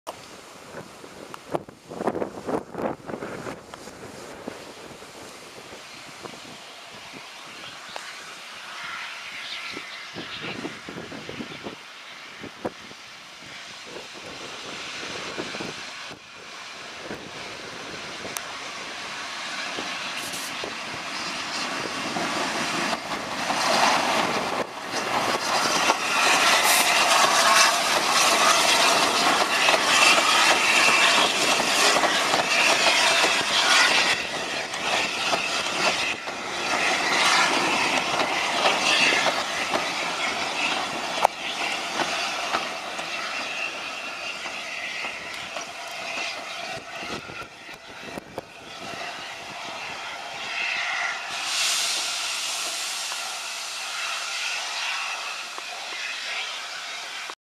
Tornado Charges tender first in Ropley Station @ Mid Hants Railway Steam gala
A1 tornado 60163 Charges into ropley station at speed tender first